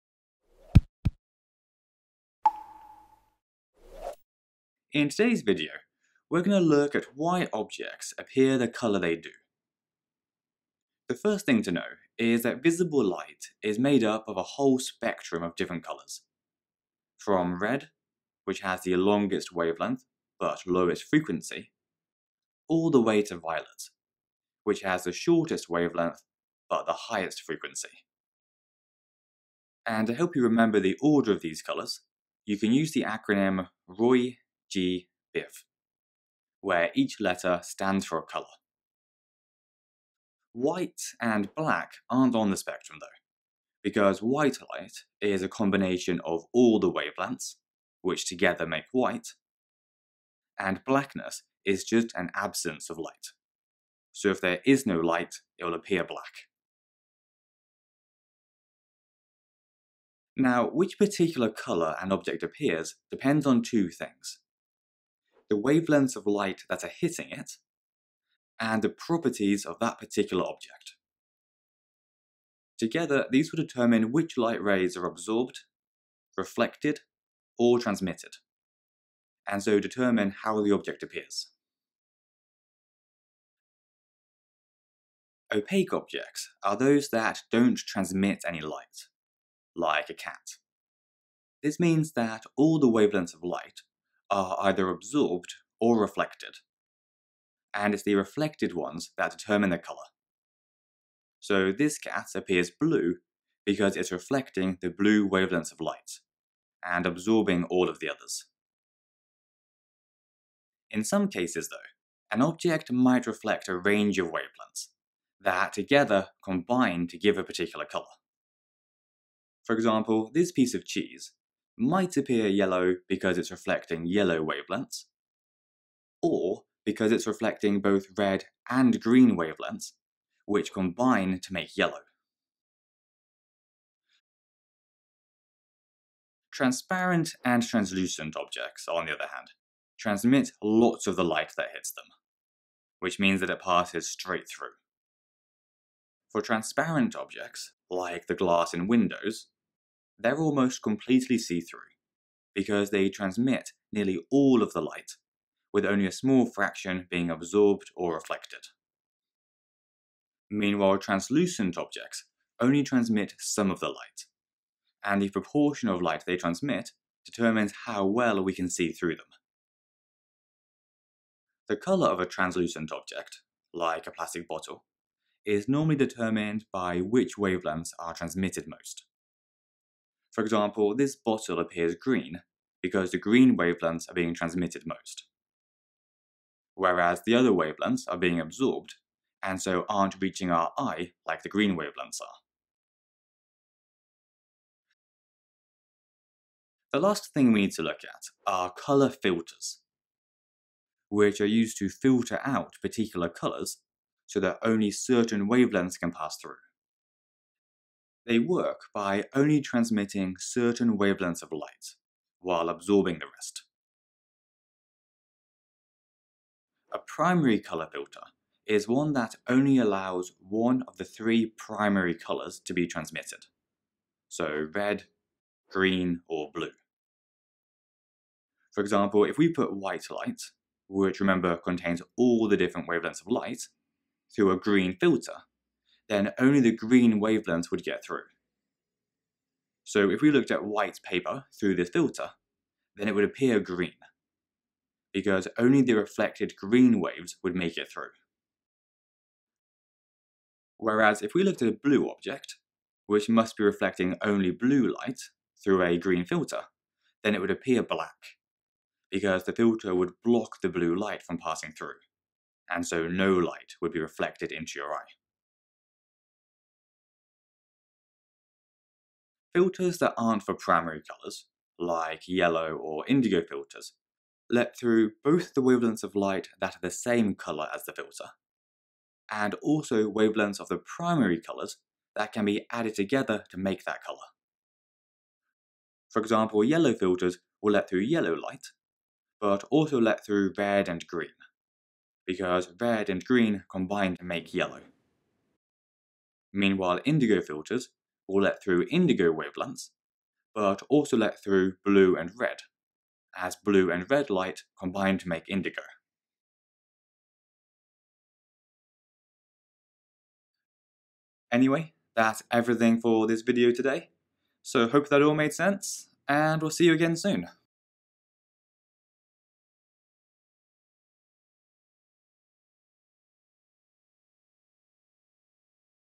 GCSE Physics - Visible Light and Colour #71
This video covers:
- Why objects appear the colour they do
- What happens when light is absorbed, reflected and transmitted
- What makes objects appear opaque, transparent and translucent
- How colour filters work

Exam board specific info:
AQA - Separate/triple science only
IGCSE Edexcel - Don't need to know the details of this video, only watch for general interest :)
Edexcel - Separate/triple science only
OCR 21st Century - Separate/triple science only
OCR Gateway - Separate/triple science only